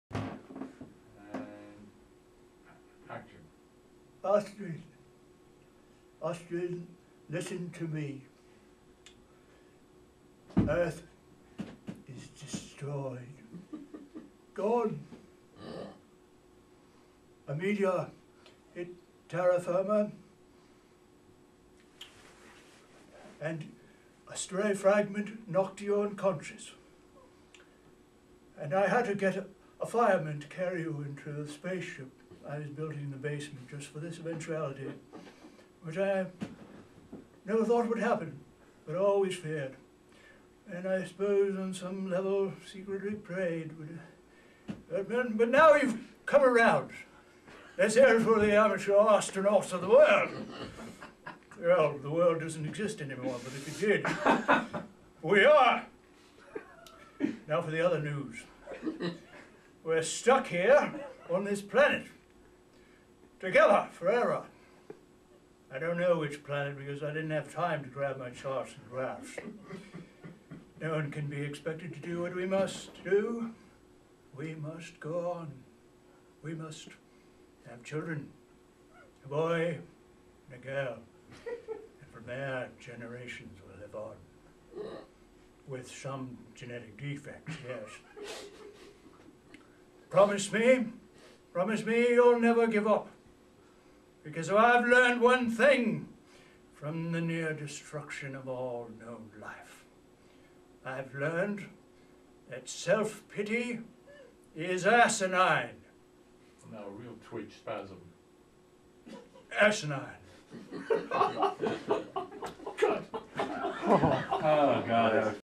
Trouble in Outer Space
Original comedy bit
Filmed at the late Cliff Osmond's acting class